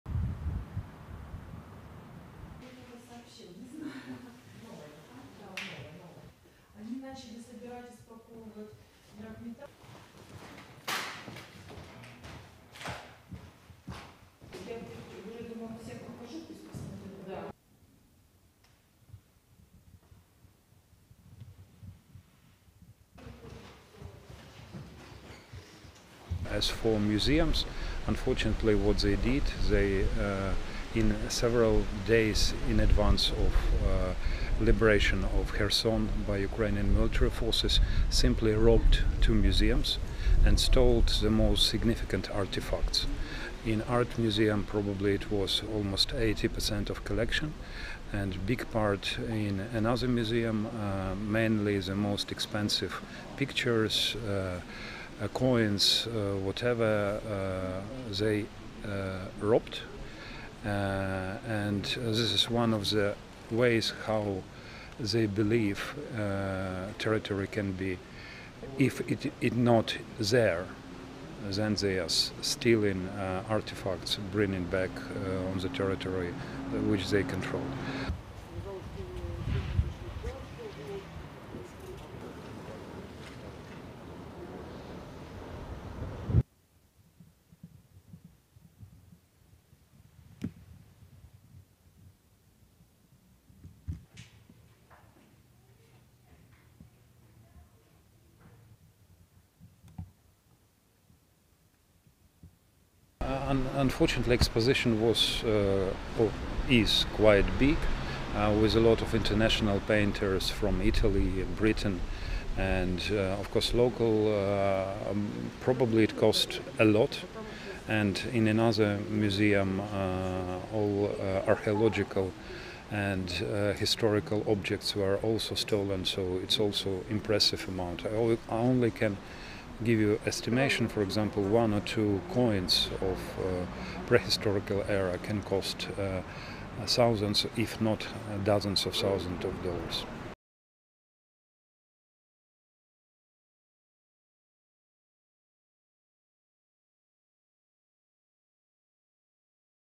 Russia looted priceless art and museum pieces from Kherson says Ukraine's Culture Minister
Russian troops retreating from Kherson looted nearly 15,000 artefacts from the Kherson Art Museum, Ukraine's Culture Minister said on Saturday (November 19).

"They (Russian forces) simply robbed two museums and stole the most significant artifacts. In Art museum, it probably was almost 80 percent of collection. And big part in another museum, mainly the most expensive pictures (paintings), coins, " Oleksandr Thachenko, Ukraine's Minister of Culture said.

The Kherson Art Museum said on Facebook a few dozen of Russian soldiers arrived at the museum on October 31 and for the next three days were seen carrying out paintings and works of art wrapped in pieces of cloth from the museum which were loaded onto trucks. According to the museum's Facebook, the looted artifacts were transferred to Simferopol in Crimea.

The collection of the Kherson Art Museum, also known as the Shovkunenko Kherson Regional Art Museum contained religious paintings dating back to the 17th century, Ukrainian art of the 19th century as well as collections of modernist and contemporary Ukrainian art.

Exposition of the Kherson Regional Museum contained some 10,000 archeological artifacts including items found during excavations of the ancient Greek colony of Olbia. The vast collection of over 173,000 artifacts stored in the museum coves the region’s natural history, pre and post revolutionary history.

To strenghten its presence in the occupied Kherson Russian authorities attempted to turn the city's regional theatre into a Russian cultural centre but with very limited success.